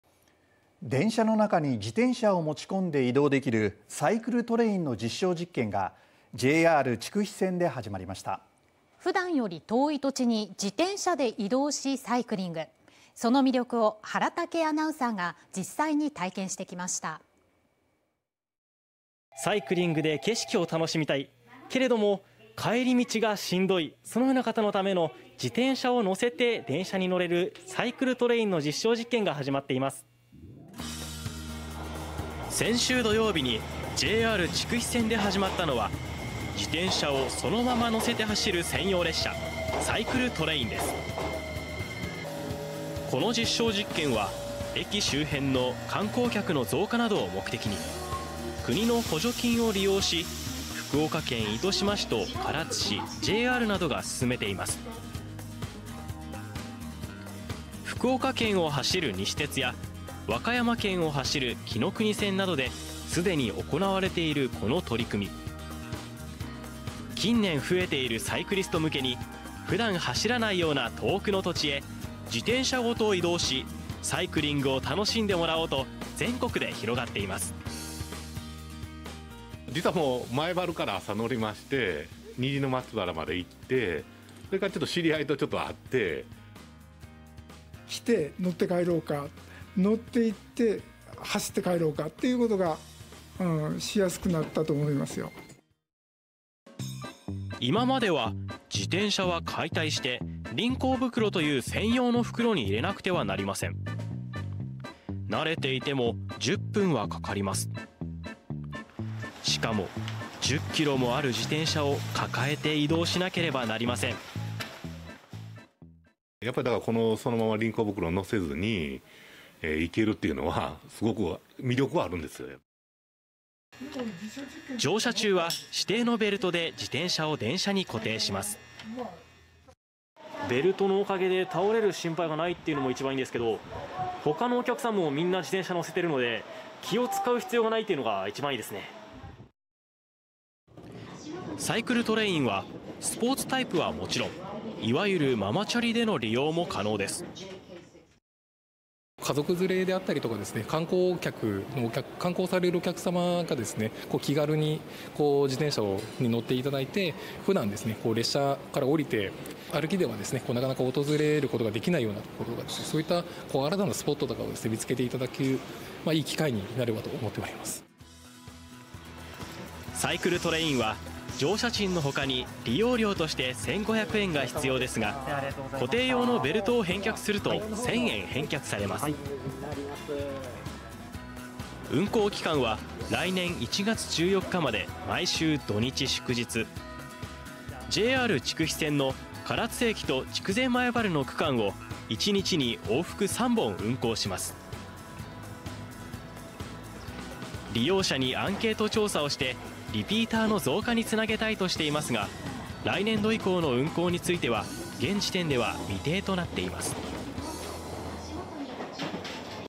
電車の中に自転車を持ち込み移動？「サイクルトレイン」実証実験がJR筑肥線で始まる【佐賀県】 (22/11/21 18:50)
電車の中に自転車を持ち込んで移動できる「サイクルトレイン」の実証実験がJR筑肥線で始まりました。
普段より遠い土地に自転車で移動しサイクリング。その魅力を原竹アナウンサーが実際に体験してきました。

【原竹アナウンサー】
「サイクリングで景色を楽しみたい、けれども、帰り道がしんどい。そんな方のための自転車を乗せて電車に乗れるサイクルトレインの実証実験が始まっています」

先週土曜日（19日）にJR筑肥線で始まったのは、自転車をそのまま乗せて走る専用列車「サイクルトレイン」です。
この実証実験は駅周辺の観光客の増加などを目的に、国の補助金を利用し福岡県糸島市と唐津市、JRなどが進めています。
福岡県を走る西鉄や和歌山県を走るきのくに鉄道などですでに行われているこの取り組み。近年増えているサイクリスト向けに普段走らないような遠くの土地へ自転車ごと移動しサイクリングを楽しんでもらおうと、全国で広がっています。

【乗客】
「実はもう前原から朝乗りまして、虹の松原まで行ってそれから知り合いとちょっと会って」
「来て、乗って帰ろうか、乗って行って走って帰ろうかということがしやすくなったと思います」

いままでは自転車は解体して、「輪行袋」という専用の袋に入れなくてはなりません。慣れていても10分はかかります。しかも10キロもある自転車を抱えて移動しなければなりません。

【乗客】
「やっぱりそのまま輪行袋乗せずに行けるというのはすごく魅力はあるんですよね」

乗車中は、指定のベルトで自転車を電車に固定します。

「ベルトのおかげで倒れる心配がないというのも一番良いんですけど、他のお客さんも皆自転車を乗せているので気を遣う必要が無いというのが一番いいですね」

サイクルトレインはスポーツタイプはもちろん、いわゆるママチャリでの利用も可能です。

【JRサイクルトレイン担当者 河野和久運輸主任】
「家族連れであったりですね、観光客、観光されるお客様が、気軽に自転車を乗っていただいて、普段、列車からおりて歩きでは中々訪れることが出来ないような所、そういった新たなスポットとかを出すね見つけて頂く良い機会になれば良いと思っております」

サイクルトレインは、乗車賃の他に利用料として1500円が必要ですが、固定用のベルトを返却すると、1000円返却されます。
運行期間は、2023年1月14日まで毎週土日祝日、JR筑肥線の唐津駅と筑前前原の区間を1日に往復3本運行します。利用者にアンケート調査をしてリピーターの増加につなげたいとしていますが、2023年度以降の運行については現時点では未定となっています。